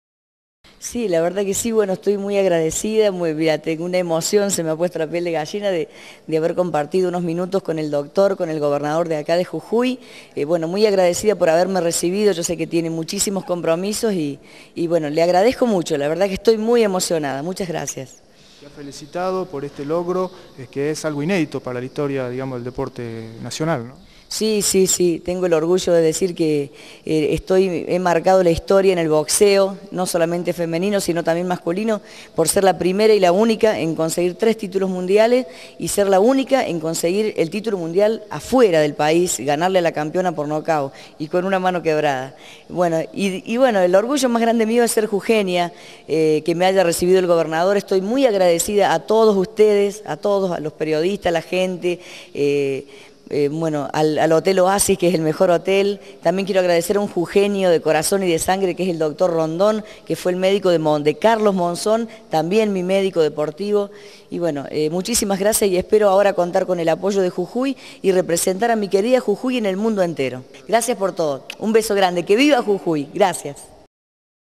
NOTA LOCOMOTORA OLIVERA.mpg
La tricampeona de la Argentina,Alejandra "Locomotora " Oliveras se refiere a su visita protocolar con el Gobernador de Jujuy, doctor Eduardo Fellner .-